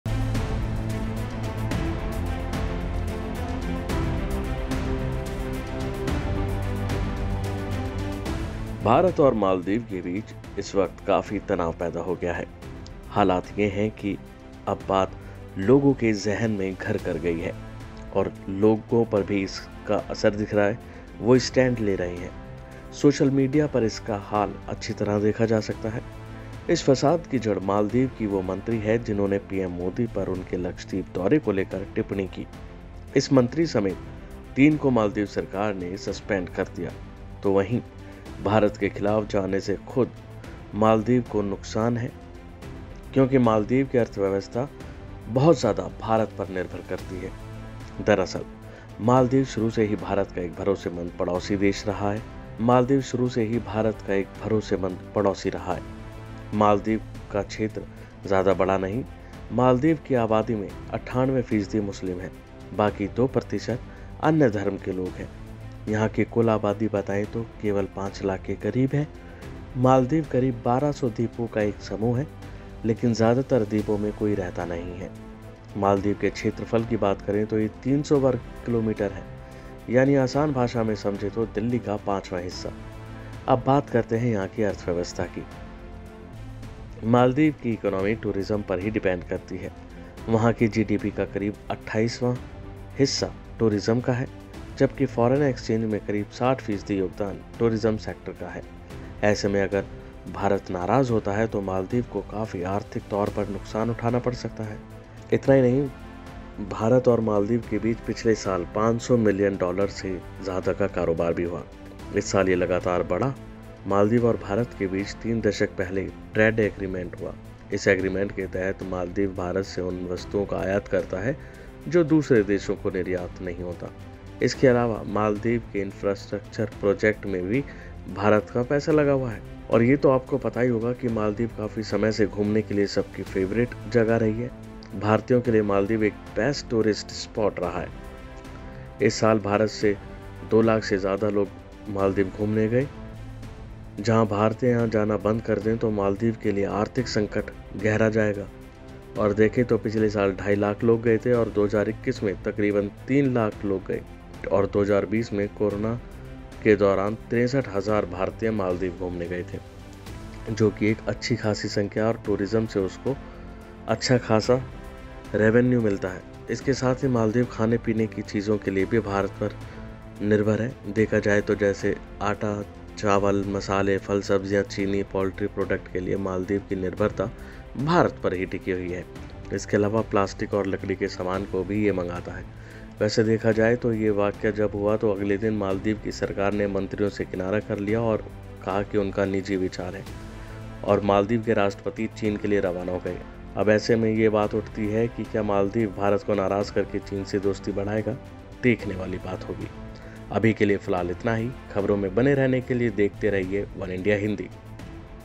India Maldives Row: अगर India ने छोड़ा साथ तो कंगाल हो जाएगा मालदीव, भारत पर निर्भर | वनइंडिया हिंदी
Boycott Maldives: भारत और मालदीव (India-maldives releation) के बीच इस बीच काफी तनाव पैदा हो गया है। हालात ये हैं कि बात अब लोगों के जहन में घर कर गई है और लोग भी इसपर स्टेंड ले रहे हैं। सोशल मीडिया पर इसका हाल अच्छी तरह देखा जा सकता है। इस फसाद की जड़ मालदीव की वो मंत्री हैजिन्होंने पीएम मोदी पर उनके लक्षदीप (PM Modi in Lakshadweep) दौरे को लेकर टिप्पणी की। इस मंत्री समेत तीन को मालदीव सरकार ने सस्पेंड कर दिया है तो वहीं भारत के खिलाफ जाने से खुद मालदीव का नुकसान है। क्योंकि मालदीव कि इकॉनोमी (maldives depend on india) भारत पर बहुत (Maldives economy depen on india) ज्यादा निर्भर करती है।